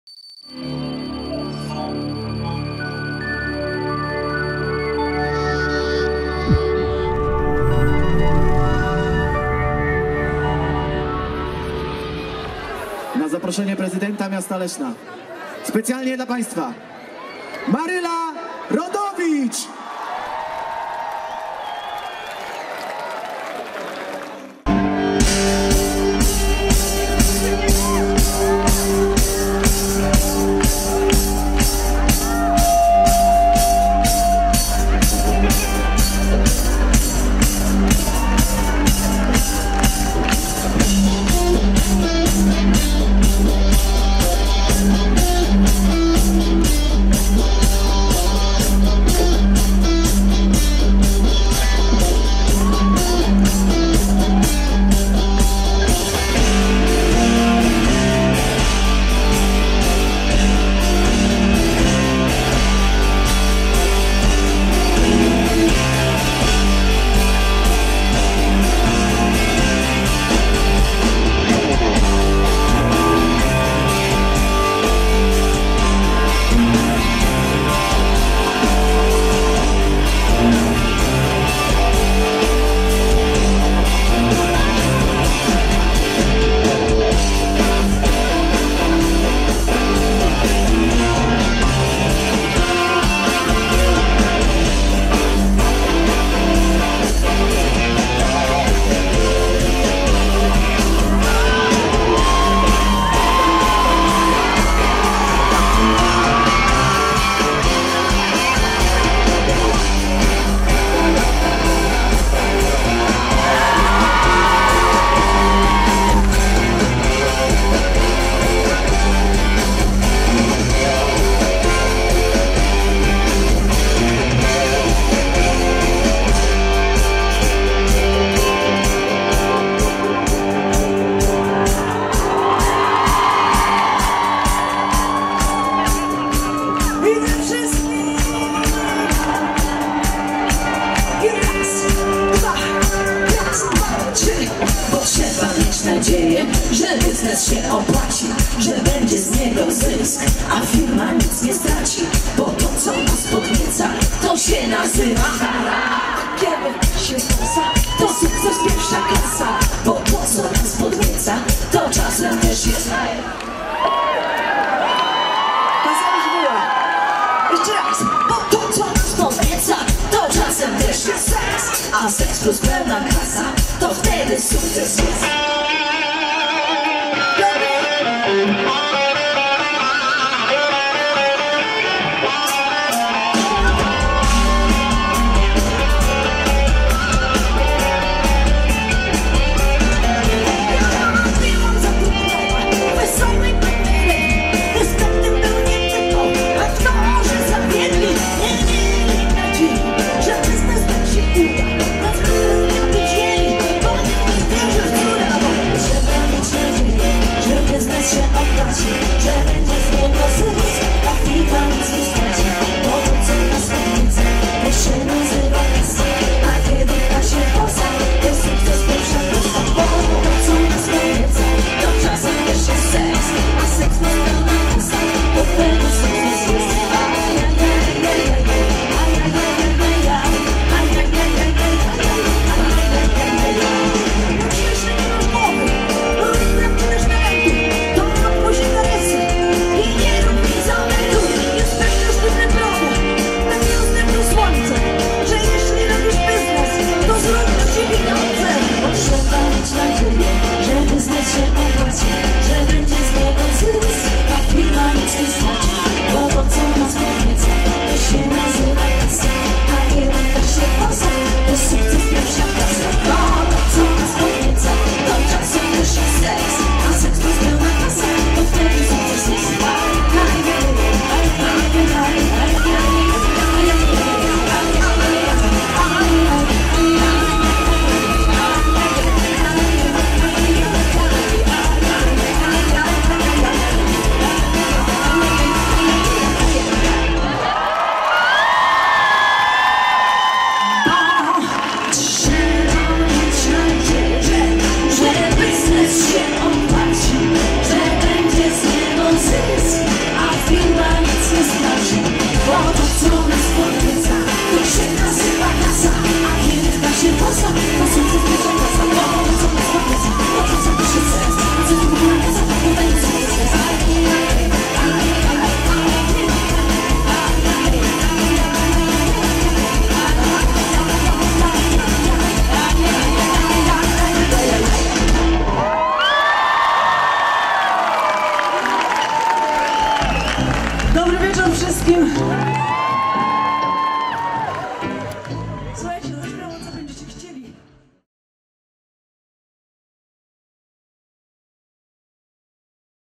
Dni Leszna - Maryla Rodowicz
Królowa polskiej muzyki Maryla Rodowicz odwiedziła Leszno, gdzie dała nie zapomniany koncert w ramach Dni Leszna 2015.